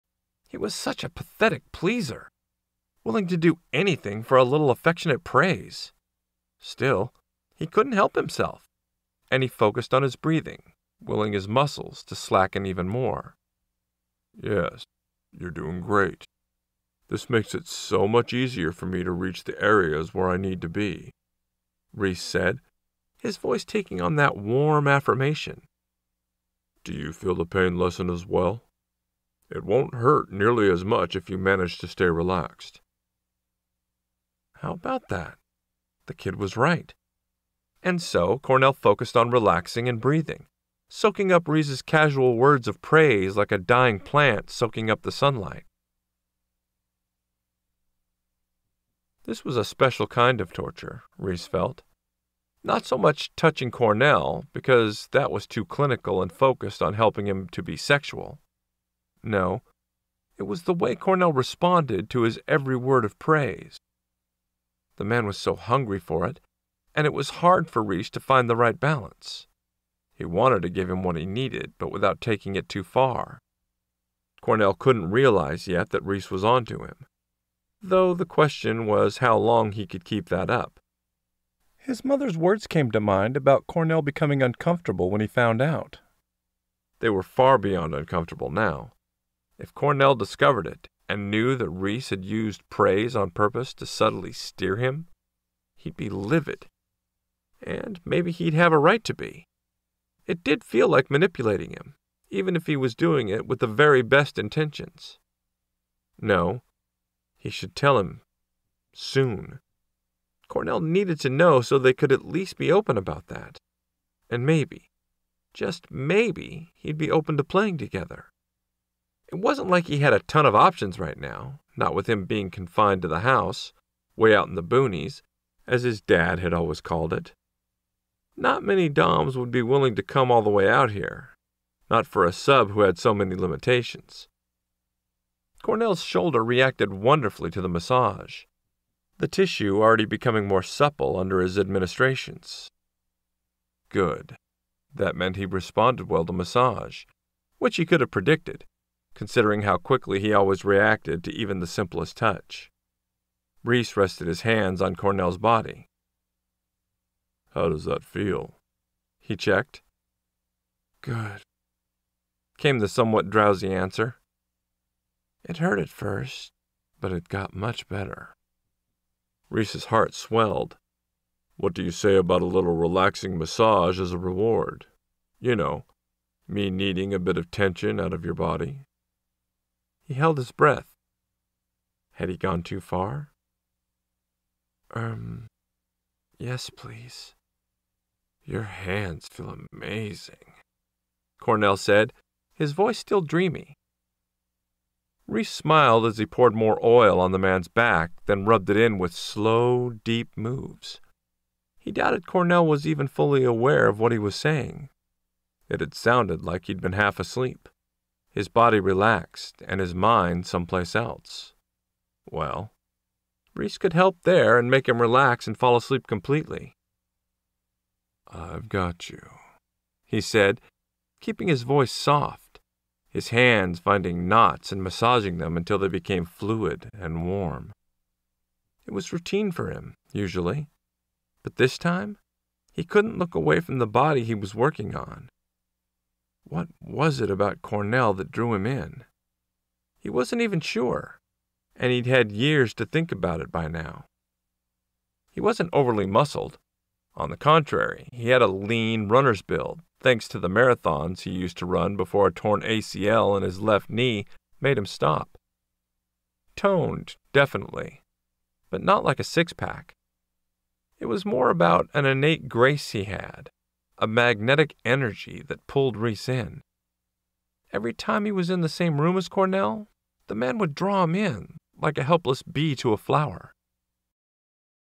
Firm Hand (Perfect Hands Book 1) by Nora Phoenix MM Romance Audiobook
MM romance audiobook, gay romance
*Firm Hand (Perfect Hands Book 1) by Nora Phoenix*
*Narrated by Kenneth Obi*

Publisher's Summary
*Cornell is broken after losing his best friend Jonas and getting hurt himself. He’s too old, too imperfect to ever attract a Dom again.*

Until Rhys.

When Rhys gives him commands, Cornell yearns to obey.

When Rhys takes care of him, Cornell wants to kneel for him. When Rhys is everything he’s dreamed of, Cornell aches to call him Sir. And when Rhys makes him fly, Cornell soars higher than ever before.

But there’s one problem. Rhys is 20 years old and is a junior...and he’s Jonas’s son. How can you want to call your best friend’s son "Sir"...or "Daddy"?

_Firm Hand is a standalone slow burn MM romance with daddy kink, an age gap, a very caring Daddy, and a boy who needs it, hurt/comfort, mild D/s play, and all the feels._